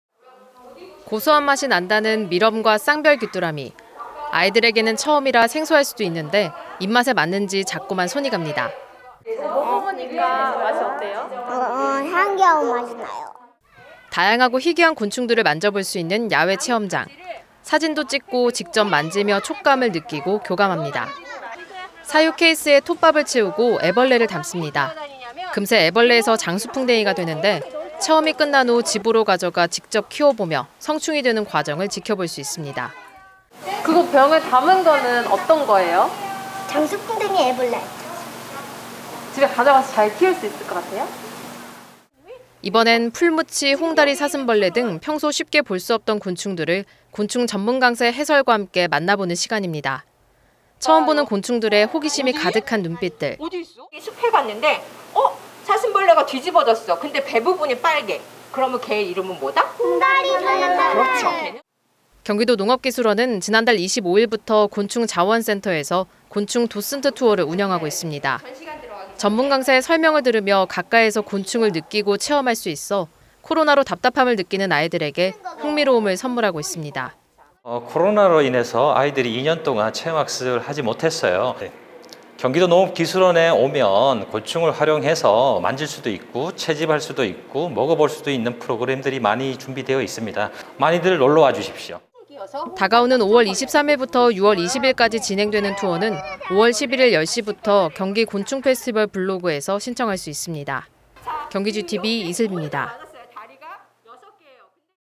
맛보고 만져보는 꿈틀꿈틀 곤충체험
[리포트]
고소한 맛이 난다는 밀웜과 쌍별귀뚜라미.
아이들에게는 처음이라 생소할 수도 있는데 입맛에 맞는지 자꾸만 손이 갑니다.

[인터뷰] 연성준 / 경기도 수원시
먹어보니까 어때요? / 참기름 맛이 나요

다양하고 희귀한 곤충들을 만져볼 수 있는 야외 체험장. 사진도 찍고 직접 만지며 촉감을 느끼고 교감합니다.

사육케이스에 톱밥을 채우고, 애벌레를 담습니다.
금세 애벌레에서 장수풍뎅이가 되는데
체험이 끝난 후 집으로 가져가 직접 키워보며 성충이 되는 과정을 지켜볼 수 있습니다.

[인터뷰] 고범준 / 경기도 수원시
병에 담은 건 뭐에요? / 장수풍뎅이 애벌레 / 집에서 잘 키울 수 있을 것 같아요?

이번엔 풀무치, 홍다리사슴벌레 등 평소 쉽게 볼 수 없던 곤충들을
곤충전문강사의 해설과 함께 만나보는 시간입니다. 처음 보는 곤충들에 호기심이 가득한 눈빛들.

[현장음] 숲에 갔는데 사슴벌레가 뒤집어 졌어 근데 배 부분이 빨개 그러면 걔 이름은 뭐다? / 홍다리사슴벌레 / 그렇죠

경기도농업기술원은 지난 달 25일부터 곤충자원센터에서 ‘곤충도슨트투어’를 운영하고 있습니다.

전문 강사의 설명을 들으며 가까이에서 곤충을 느끼고 체험할 수 있어
코로나로 답답함을 느끼는 아이들에게 흥미로움을 선물하고 있습니다.

[인터뷰] 김영탁 / 경기도농업기술원 농촌지도사
코로나로 인해서 아이들이 2년 동안 체험학습을 하지 못했어요.
경기도농업기술원에 오면 곤충을 활용해서 만질 수도 있고 채집할 수도 있고 먹어볼 수도 있는 프로그램들이 많이 준비돼 있습니다.
많이 놀러와 주십시오.

다가오는 5월 23일부터 6월 20일까지 진행되는 투어는
5월 11일 10시부터 경기곤충페스티벌 블로그에서 신청할 수 있습니다.

영상취재: 이민준  영상편집: 윤지성

[자막]

1. 식용곤충 시식체험

2. [인터뷰] 연성준 / 경기도 수원시

3. 곤충 만져보기, 애벌레 채집 등 이색 체험

4. [인터뷰] 고범준 / 경기도 수원시

5. 곤충생태온실 관람

6. [현장음]

7. 25일부터 곤충자원센터에서 ‘곤충도슨트투어’ 운영

8. [인터뷰] 김영탁 / 경기도농업기술원 농촌지도사

9. 경기곤충페스티벌

10. 영상취재: 이민준  영상편집: 윤지성